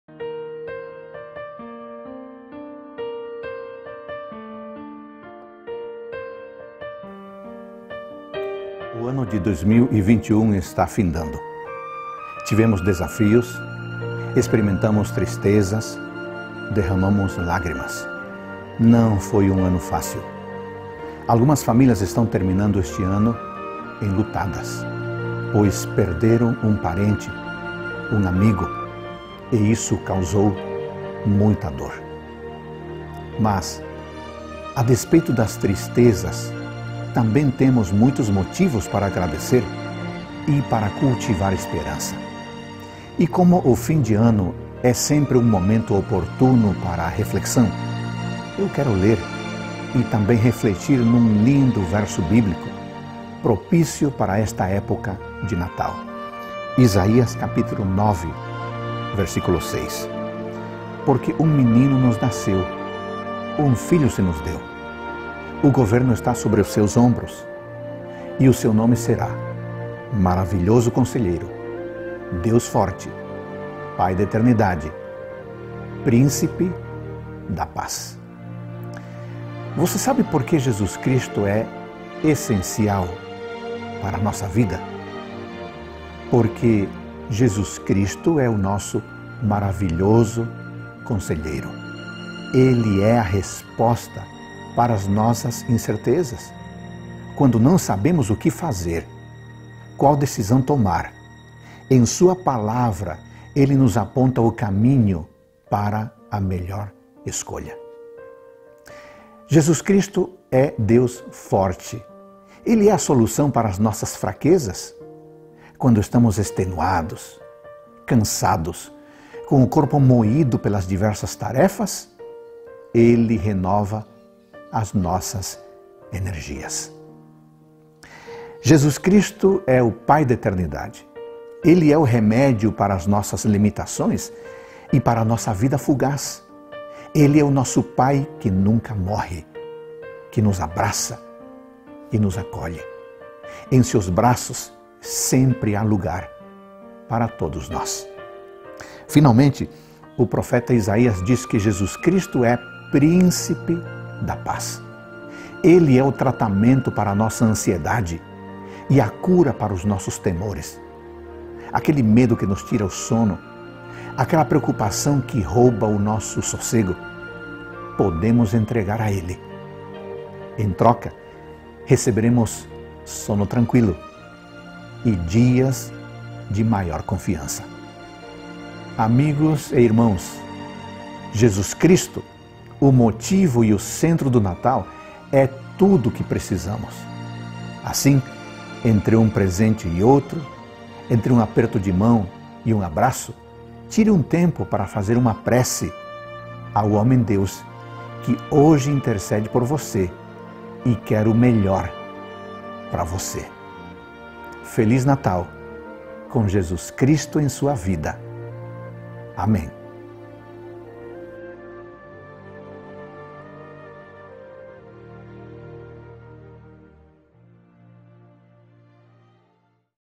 Feliz Natal - rpsp
Desejo a todos vocês e suas famílias, um Feliz Natal e um ano 2022 repleto das bênçãos de Deus. Abraços - Maria Eduarda